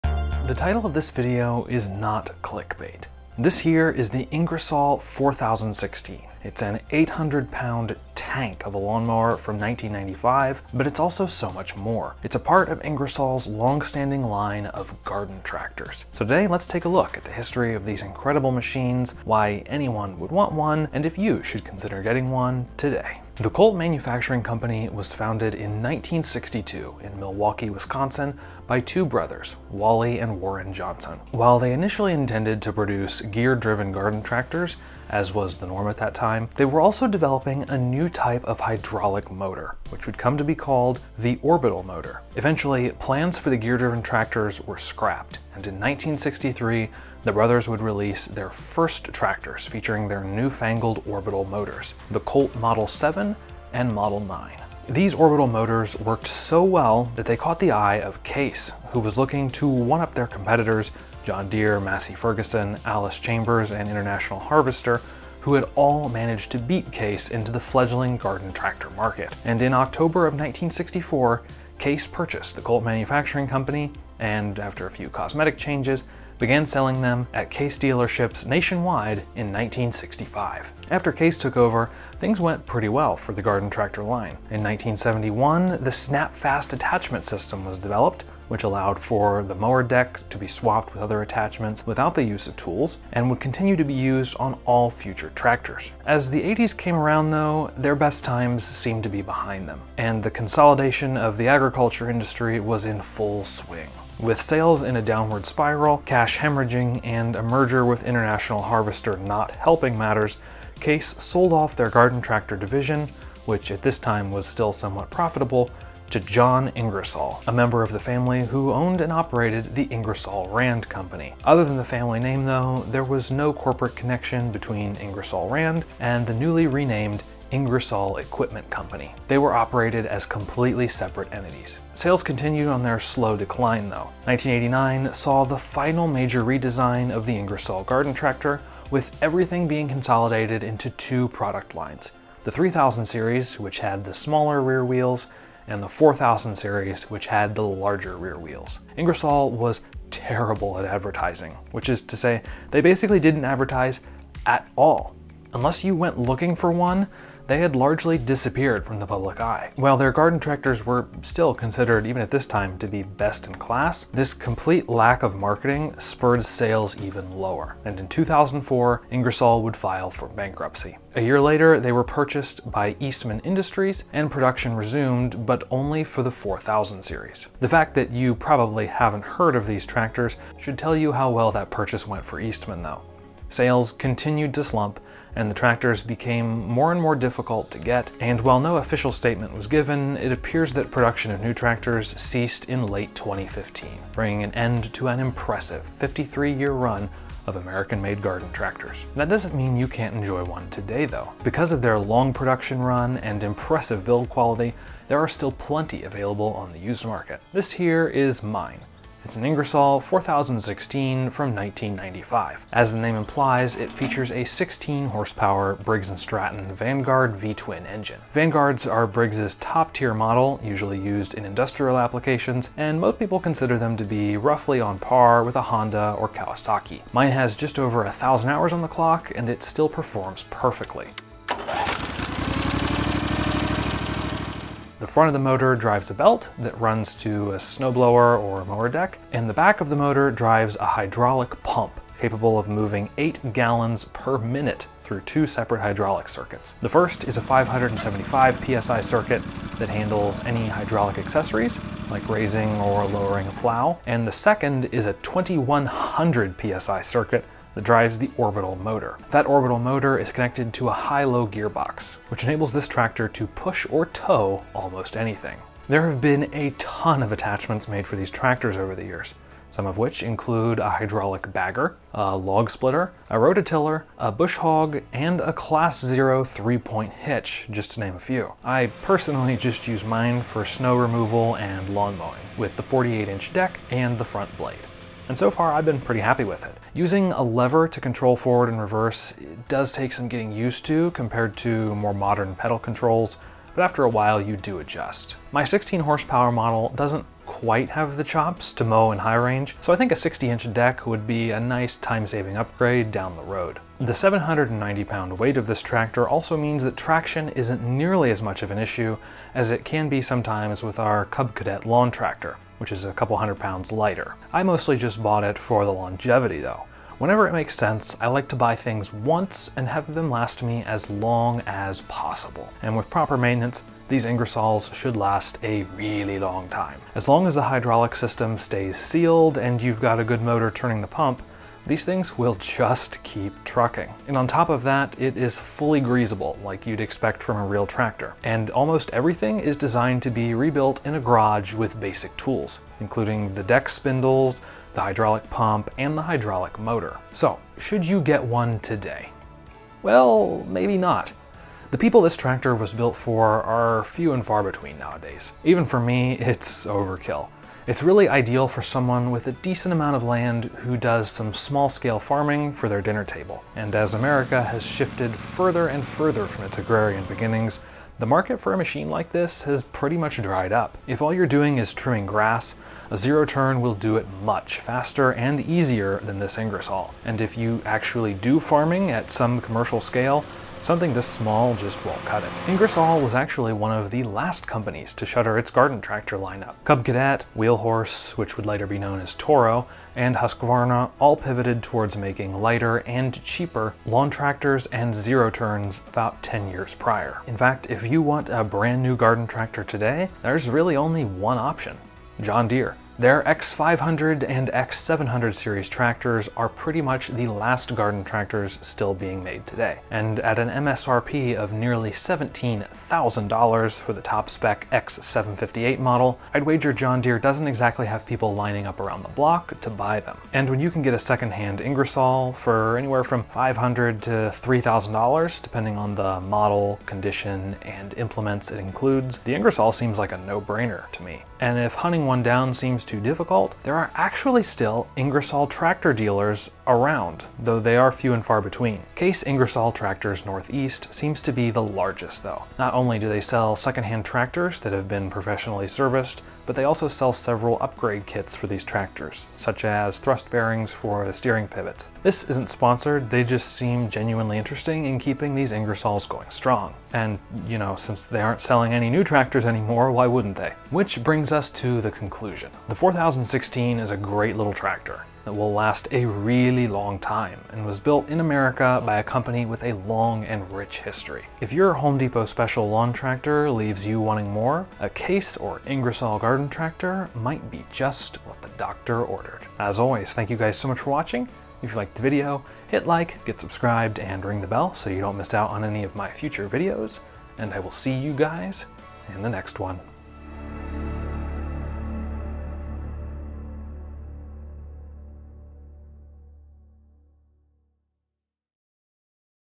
This Lawn Mower Will Outlive Me, You, and Everyone You Know - Ingersoll 4016 Review
Today lets take a look at the Ingersoll 4016, and dig into the long history of these iconic garden tractors...

Gear Used:
Camera: Fujifilm X-H1 & X-H2S
Lens: Fujifilm XF 35mm f/1.4, Fujifilm XF 18-55mm f/2.8-4
Mic: Deity D3 Pro
Editing Software: Final Cut Pro X